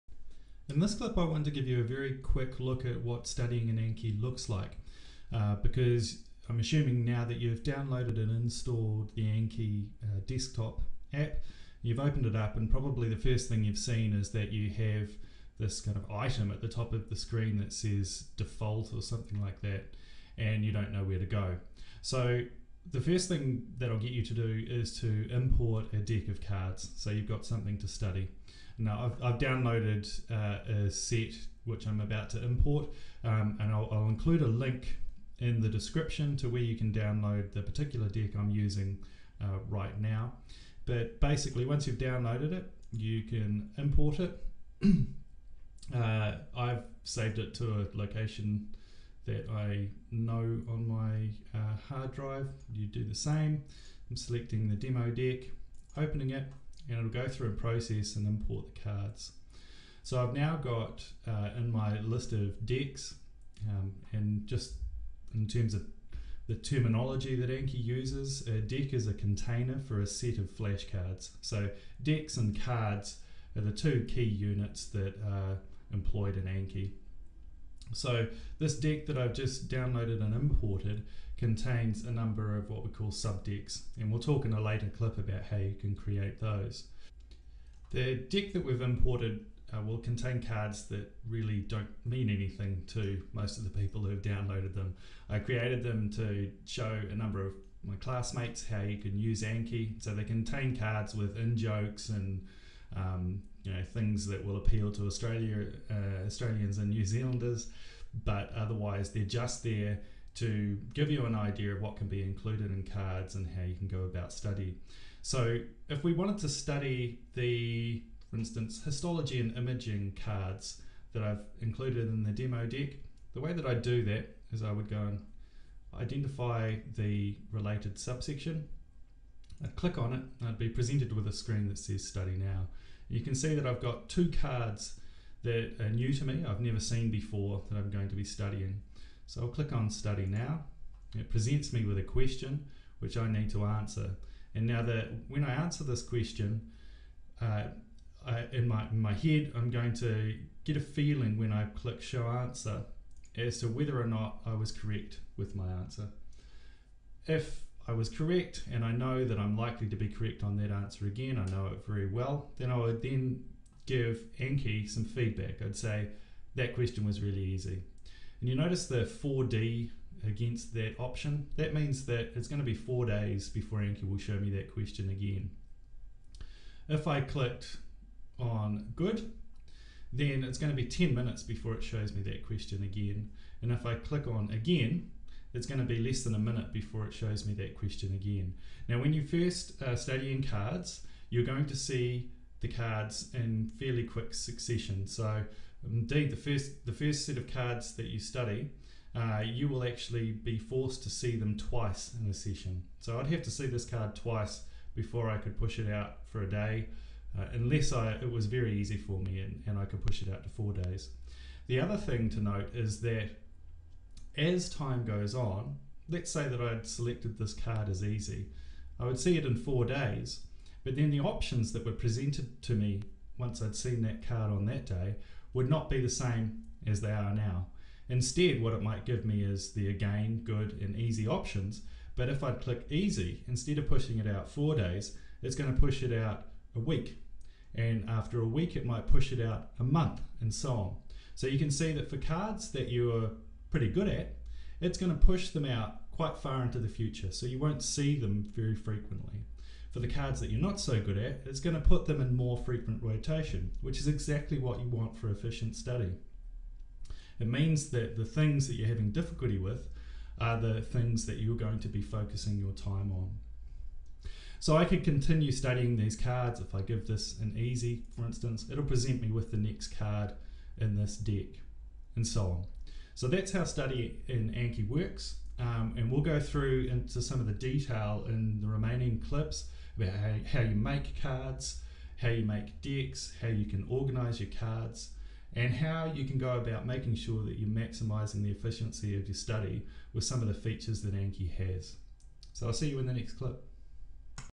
S01 - Anki How-to Series: 03 Studying your first deck and some basic terminology
New to Anki?

Does the video seem blurry? Switching to HD quality (in the settings) and watching in full-screen mode will fix this.

What does study in Anki look like? This clip will show you how to import your first 'deck' of flashcards and get straight into study. See how the feedback you provide to Anki will influence how frequently you see each flashcard, and why this makes study more effective.